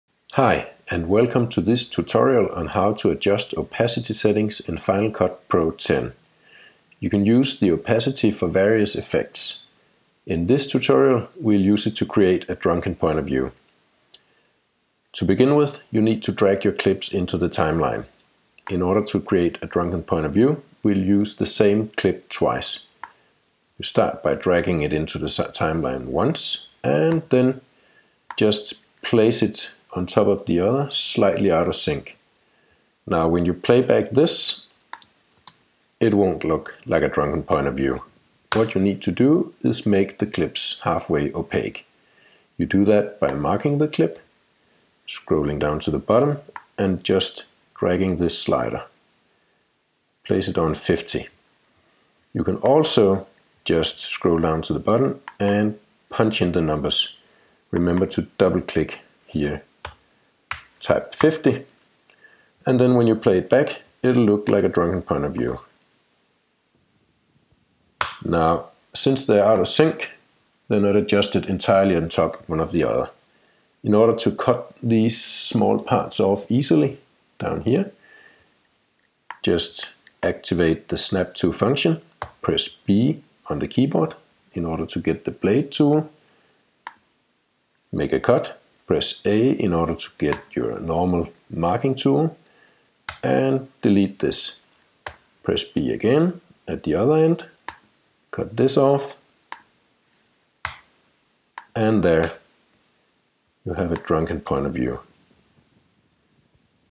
How to create drunk pov in Final cut pro X by adjusting opacity
By layering your clips on the timeline and adjusting the opacity settings of the clips, you can create a drunken looking point of view in fcpx. This tutorial shows you how.